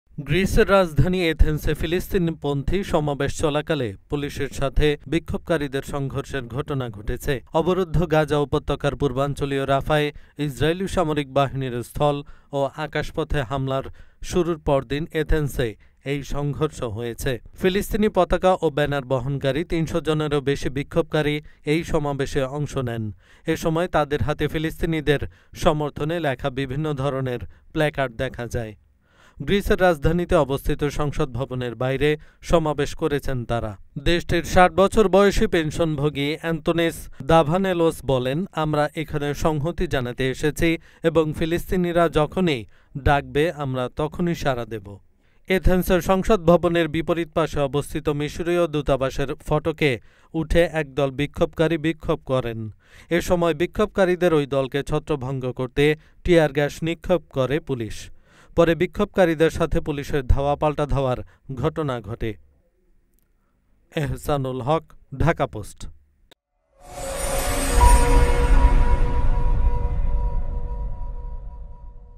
গ্রিসে পুলিশের সাথে ফিলিস্তিনপন্থি বিক্ষোভকারীদের সংঘর্ষ | Latest Bangla News | Greece | Dhaka Post
গ্রিসের রাজধানী এথেন্সে ফিলিস্তিনপন্থি সমাবেশ চলাকালে পুলিশের সাথে বিক্ষোভকারীদের সংঘর্ষের ঘটনা ঘটেছে। অবরুদ্ধ গাজা উপত্যকার পূর্বাঞ্চলীয় রাফায় ইসরায়েলি সামরিক বাহিনীর স্থল ও আকাশপথে হামলা শুরুর পরদিন এথেন্সে এই সংঘর্ষ হয়েছে.